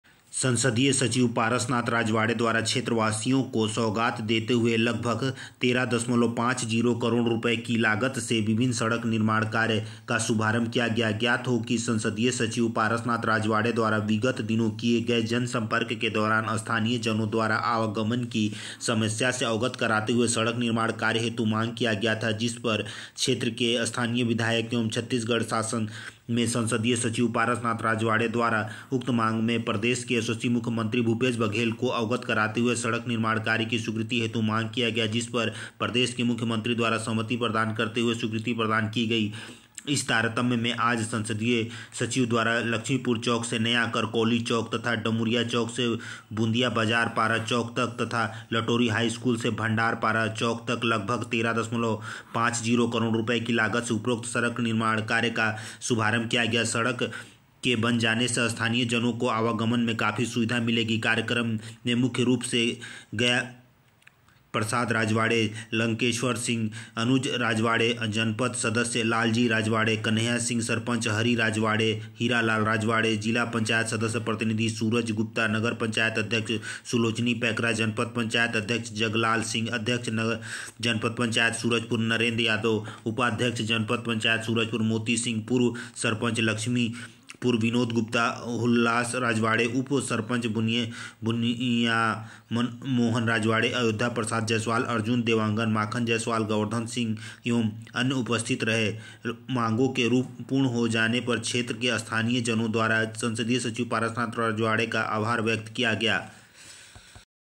संसदीय सचिव पारसनाथ राजवाड़े द्वारा लगभग 13.50 करोड़ रुपए की लागत से विभिन्न सड़क निर्माण का शुभारंभ
सूरजुपर : संसदीय सचिव पारसनाथ राजवाड़े द्वारा लगभग 13.50 करोड़ रुपए की लागत से विभिन्न सड़क निर्माण कार्य का किया गया शुभारंभ